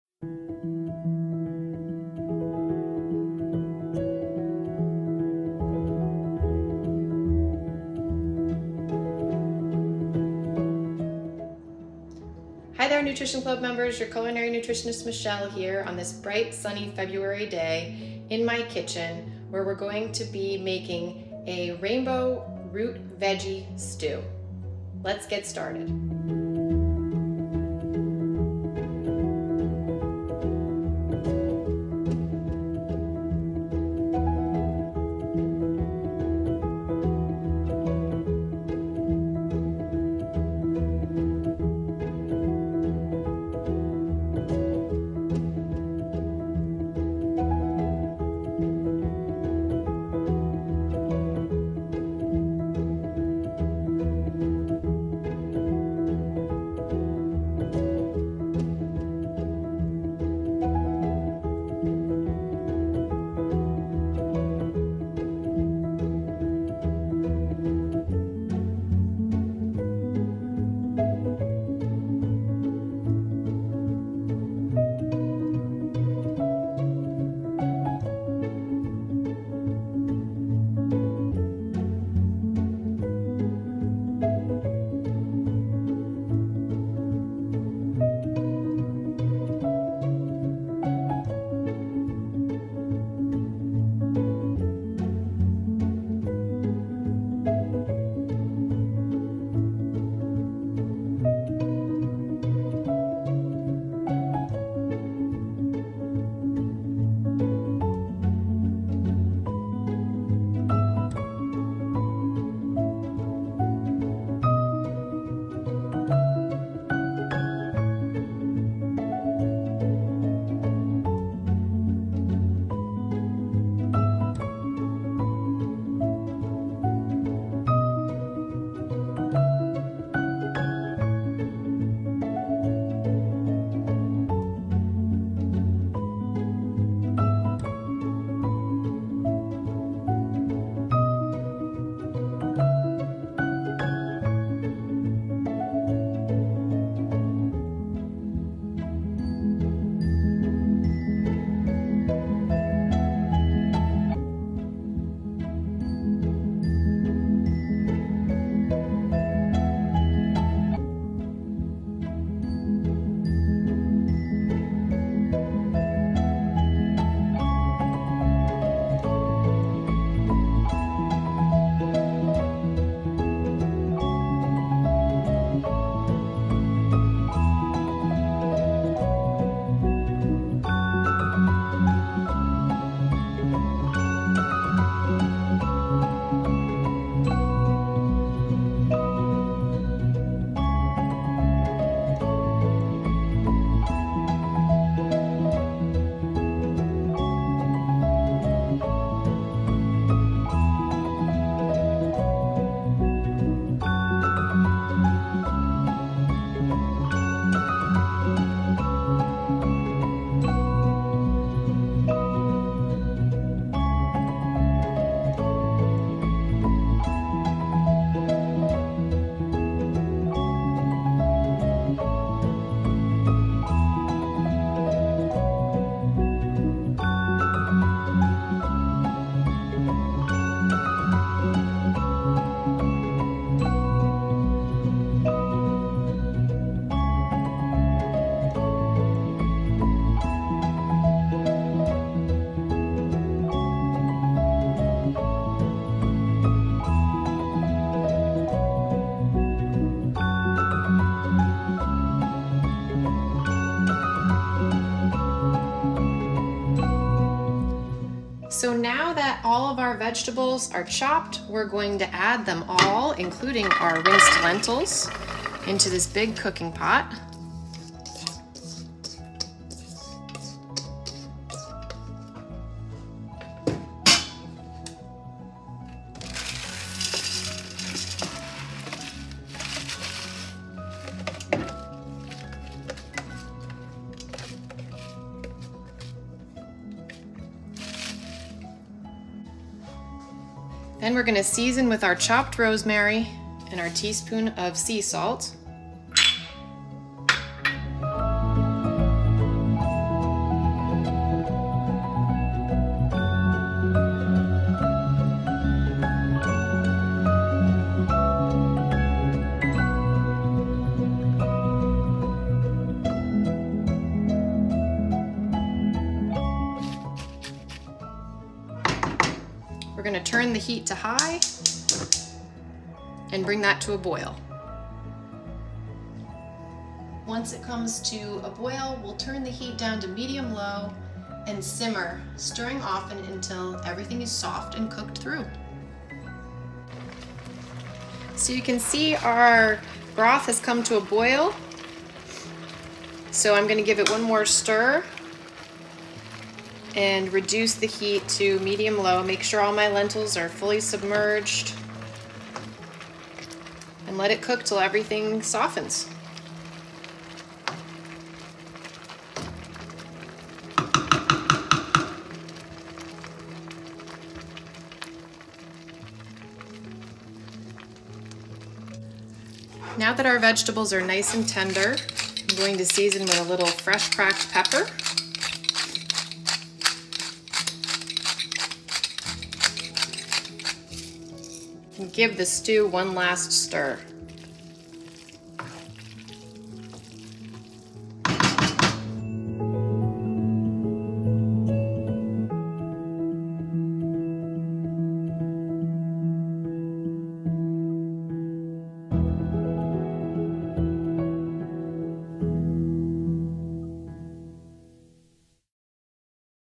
🌈 Rainbow Root Veggie Stew 🌈
Bring some color to the final weeks of winter with this rainbow root veggie stew! Ingredients and steps below...

Ingredients:
1 large yellow onion, peeled & diced
2 garlic cloves, minced (optional)
3-4 medium sweet potatoes, washed, peeled, & cubed (about 6 cups)
3 large (6 medium) beets, washed, peeled, & cubed
4 large carrots, washed, peeled, & thinly sliced
4 celery stalks, washed & thinly sliced
1 bunch spinach, washed
1 pound dry lentils, rinsed
6 cups vegetable broth
1 teaspoon sea salt
3 sprigs rosemary, washed, leaves removed & finely chopped
Black pepper, to taste
Nutritional yeast, to taste (optional)

Preparation:
Place all of the vegetables into a cooking pot (you can also use a slow cooker or pressure cooker). Season with salt and rosemary and pour in the vegetable broth. Give the ingredients a quick stir and make sure the lentils are entirely submerged.
Bring the broth to a boil over high heat; reduce heat to medium-low, and simmer, stirring often, until all of the ingredients are soft and cooked through.
Season stew to taste with black pepper and serve it topped with a sprinkle of nutritional yeast (optional).